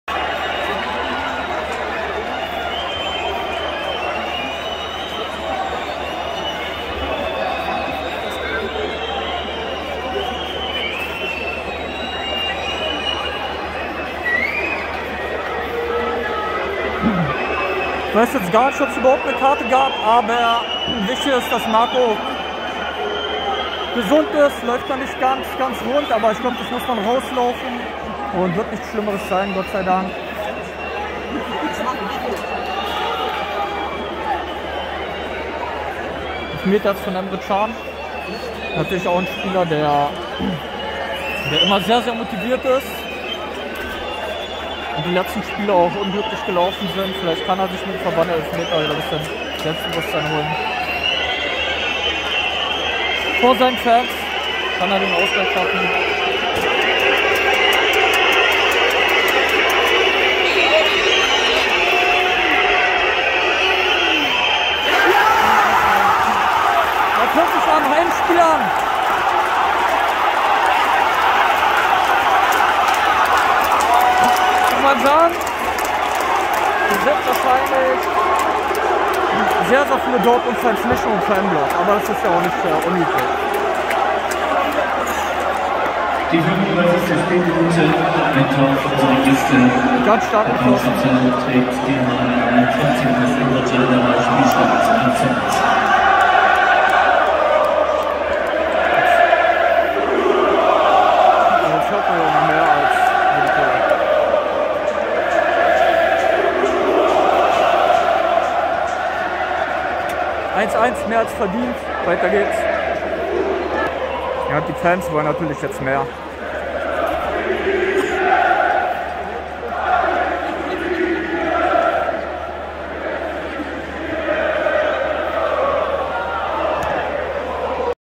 Bundesliga Highlight: Wolfsburg - Borussia Dortmund. Emre Can Elfmeter Tor zum 1:1...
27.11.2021 Volkswagen Arena. Wolfsburg - Borussia Dortmund 1:3. Emre Can trifft per Elfmeter zum 1:1 + BVB Fans Stimmung nach Tor...